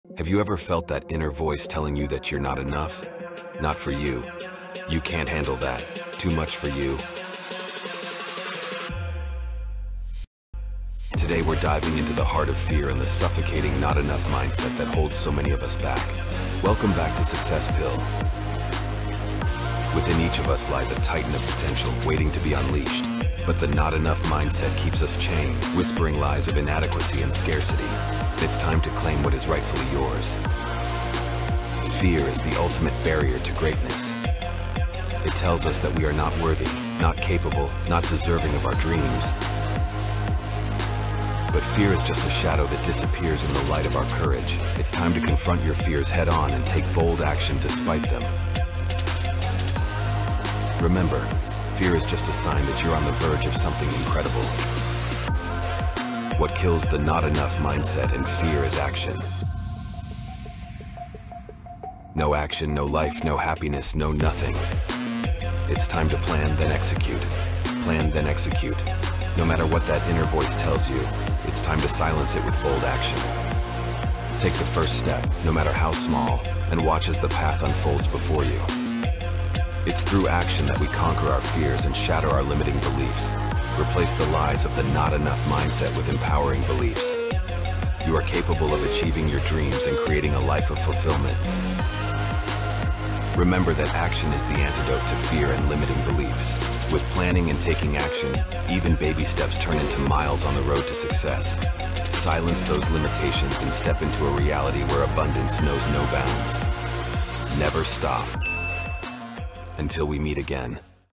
Fearless Breakthrough: Embracing true self
Welcome back to SuccessPill!
Today, we're discussing "not enough" mindset and FEAR that hold us back from going after what we want and be someone who matters in life.
It's time to break free from these limitations and unleash your inner titan!

Join us as we confront these fears head-on and take bold action towards our dreams. No more excuses, no more limitations - it's time to rewrite the narrative and step into the truth of who we are. Let's silence those doubts and embrace a reality where abundance knows no bounds.

footage:  @PIXABAYs   @PexelsPhotos